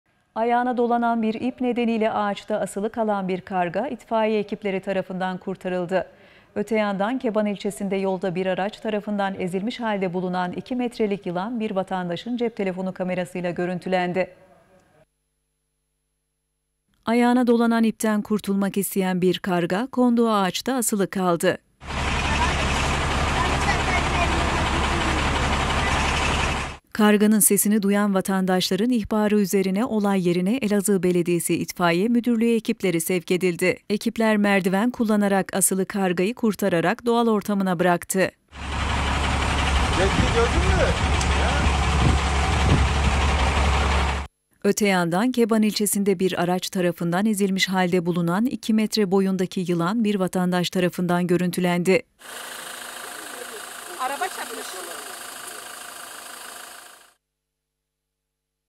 İPTEN KURTULMAK İSTERKEN AĞAÇTA ASILI KALDI - İKİ METRELİK YILAN YOLDA EZİLMİŞ OLARAK BULUNDU
AYAĞINA DOLANAN İP NEDENİYLE AĞAÇTA ASILI KALAN BİR KARGA İTFAİYE EKİPLERİ TARAFINDAN KURTARILDI. ÖTE YANDAN KEBAN İLÇESİNDE YOLDA BİR ARAÇ TARAFINDAN EZİLMİŞ HALDE BULUNAN 2 METRELİK YILAN BİR VATANDAŞIN CEP TELEFONU KAMERASI İLE GÖRÜNTÜLENDİ